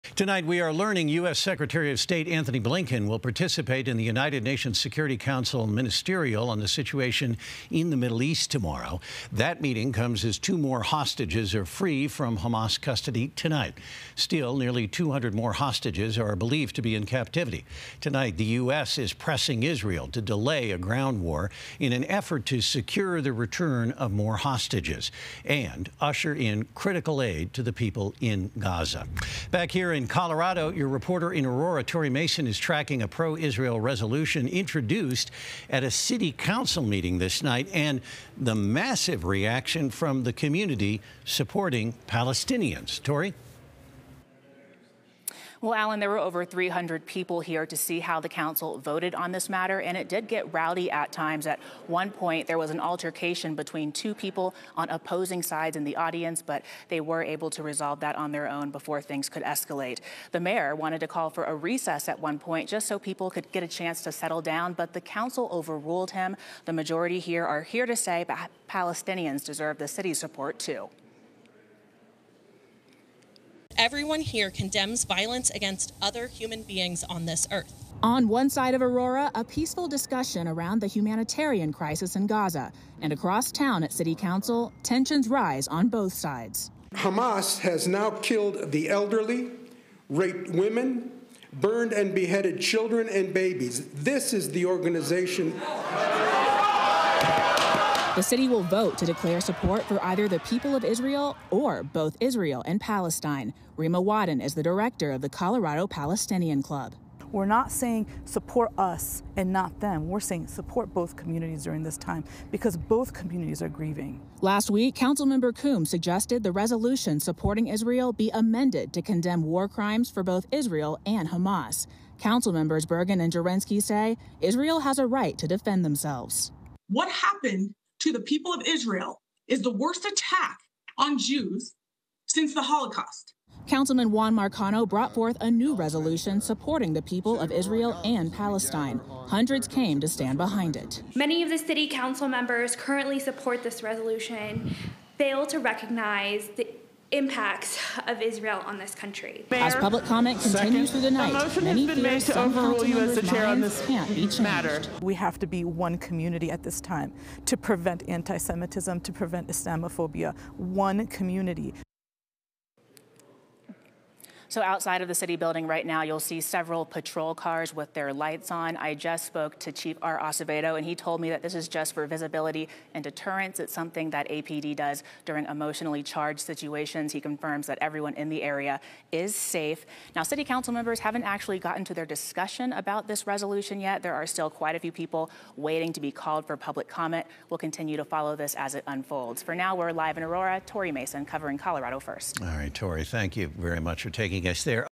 Proposed Aurora city council resolution supporting Israel prompts massive outcry from community
The majority of public speakers at Monday's council meeting said Palestinians deserve the city's support too. While a peaceful rally took place outside city hall, heated arguments erupted inside the council chambers.